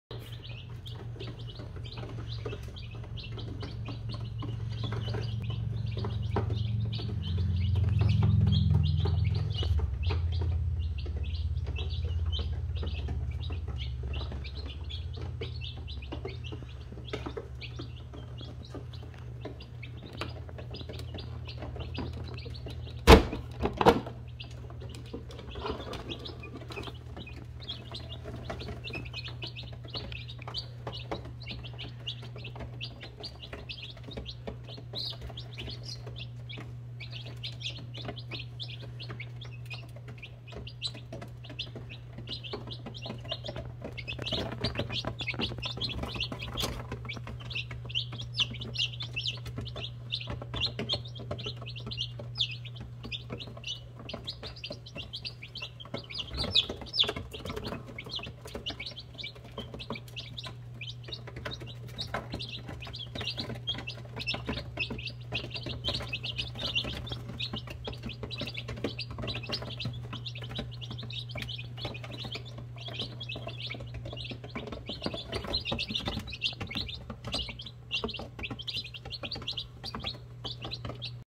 Day Old Chicks (Buff Orpington, Cream Legbar, Black Copper Marans) in Hatching Time Brooder
Day old baby chicks from Meyer's Hatchery running around their new brooder after arriving via shipping. They are just too cute!